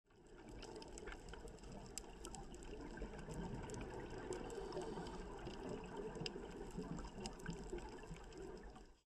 Bunaken Island: Scorpionfish
Dive into the breathtaking beauty of Bunaken National Marine Park in this teaser! 🌊🐢 Experience the full underwater adventure – Watch the complete scuba diving film now!